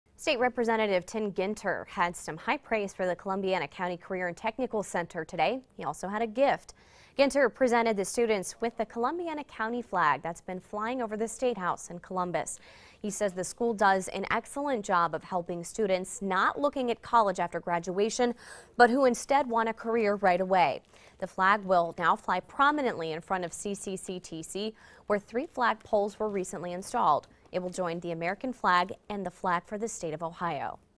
Statehouse flag presented to Columbiana County Career and Tech. Center
State Rep Tim Ginter, R-Salem, had high praise and a gift for the Columbiana County Career and Technical Center.